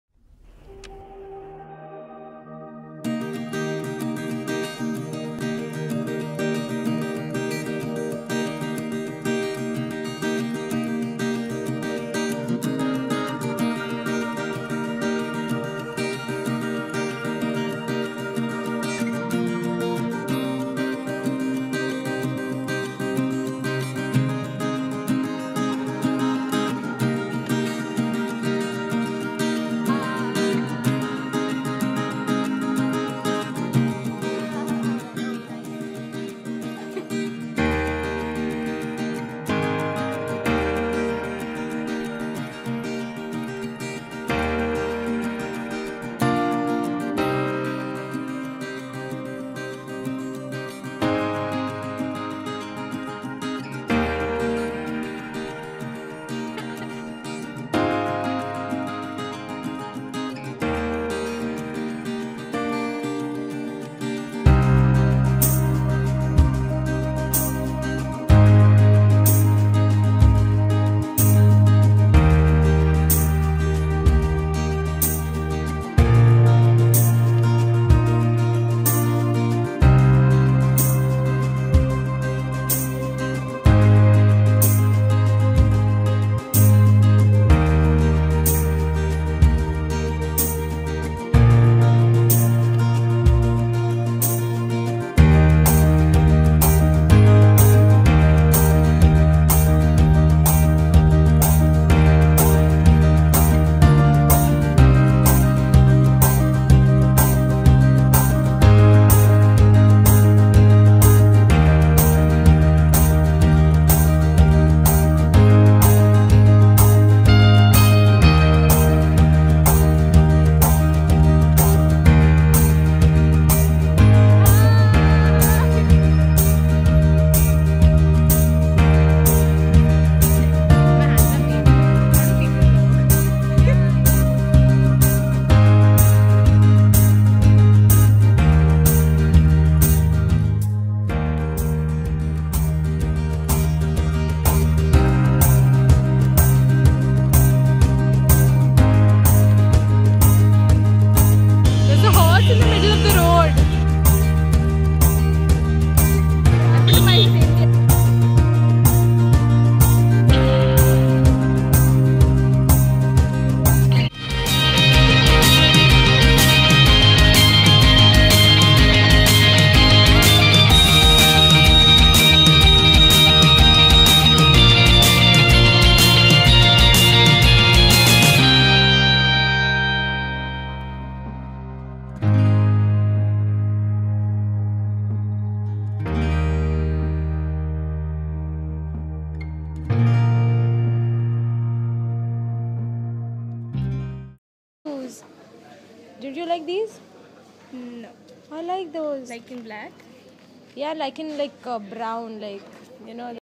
Spontaneity | Amalthya (a Kitab Khana and Mumbai Fort vlog)
We headed out to see the museum.
Lost our way, walked around town, ended up in a bookstore meanwhile only to learn that the best things happen when they aren't planned.

Like if you care.
Share this experience.

Kay byee!